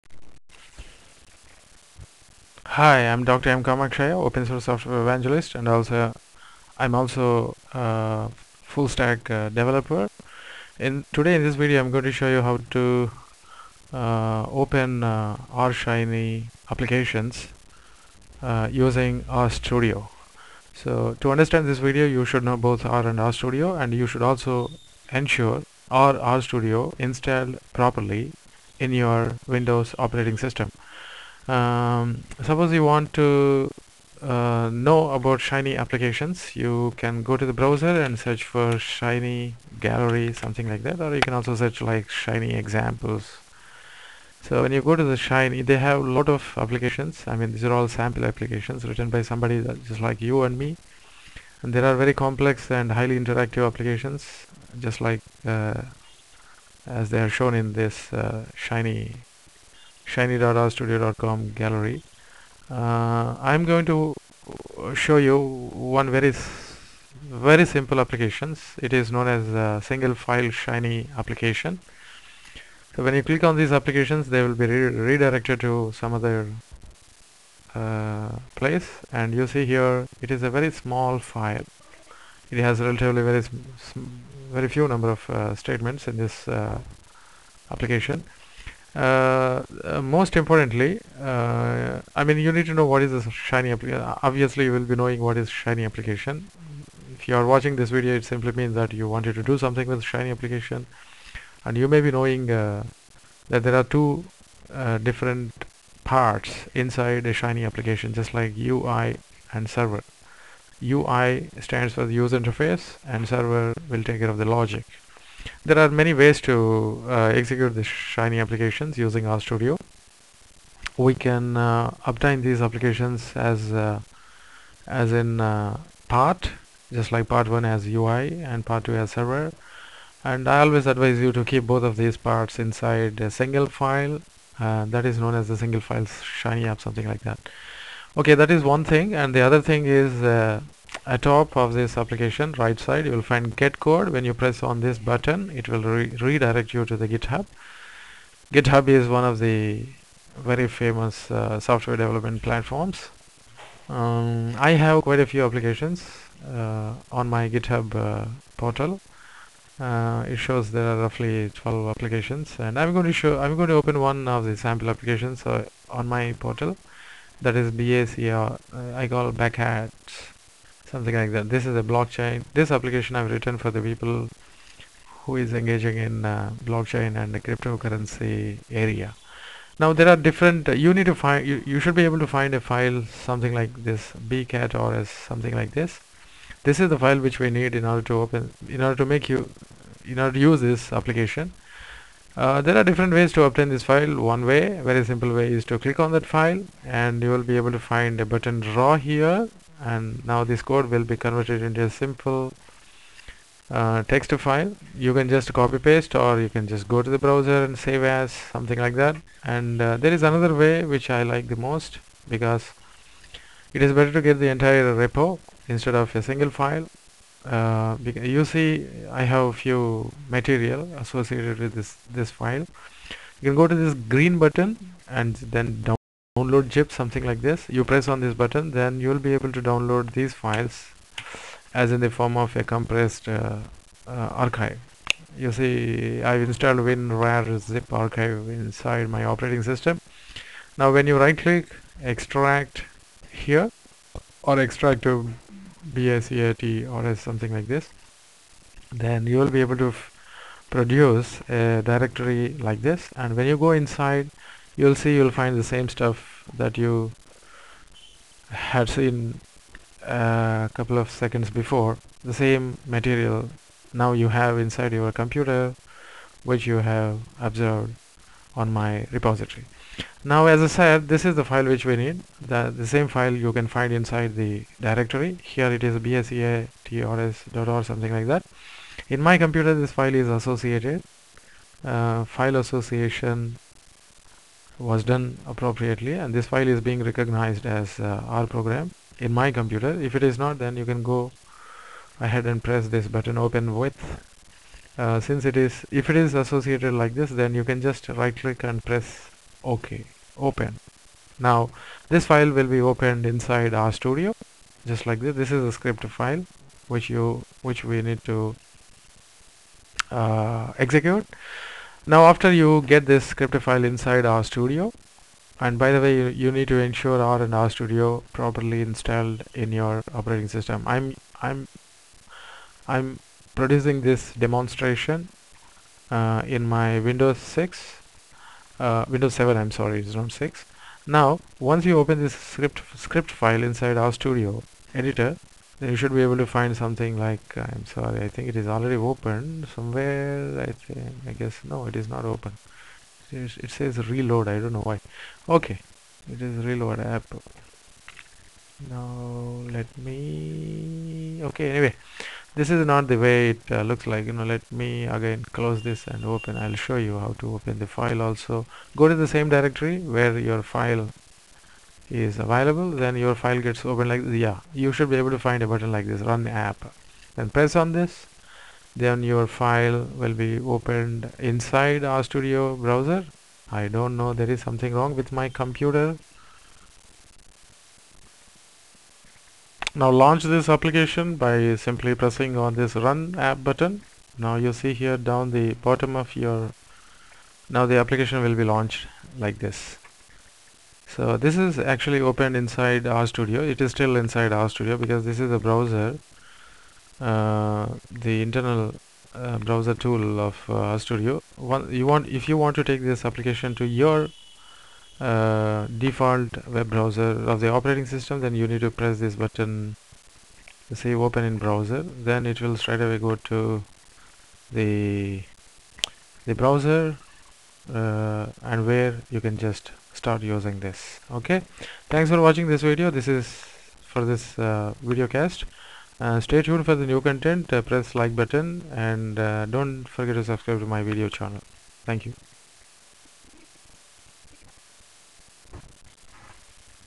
How to open R Shiny apps using R Studio
This is a video demonstration that helps to know as how to open Shiny applications using RStudio.